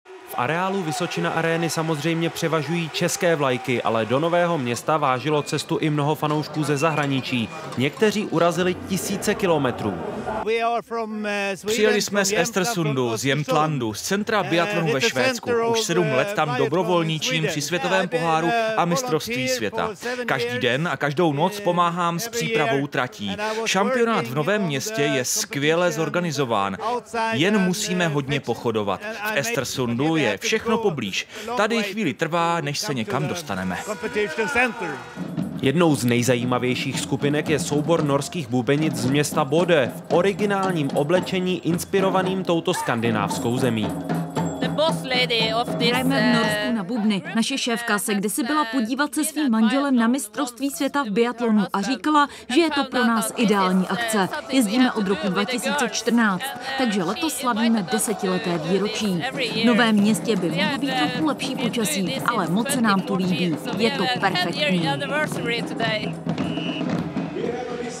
Jak se zahraničním fanouškům líbí v Novém Městě? | MS v biatlonu 2024
Ve Vysočina Aréně převažují české vlajky, ale do Nového Města na Moravě přicestovaly i tisíce zahraničních fanoušků. Jak se jim v centru českého biatlonu líbí?

Foto: Český biatlon/Petr Slavík